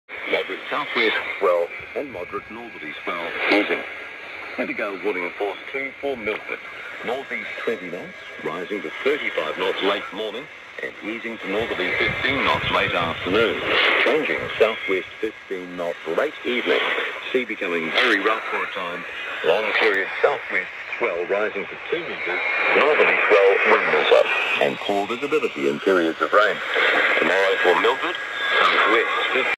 7390kHz Radio New Zealand 15:13UTC 23-Dec-2023
Strong signal from Radio New Zealand on TEF6686 and active loop (RA0SMS preamplifier).